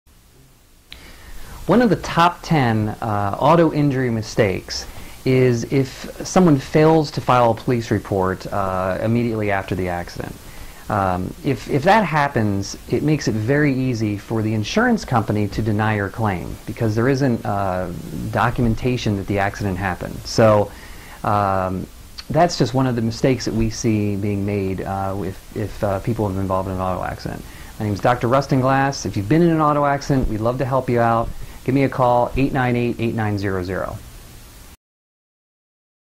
Top 10 Motorcycle Injury Mistakes No Report Chiropractor Lancaster PA Dr. Glass
Motorcycle Accident Injury Treatment, Dr. Rustin Glass Chiropractor in Lancaster Pennsylvania .

Most Health Insurance and All Auto Accident Injury Insurance Accepted.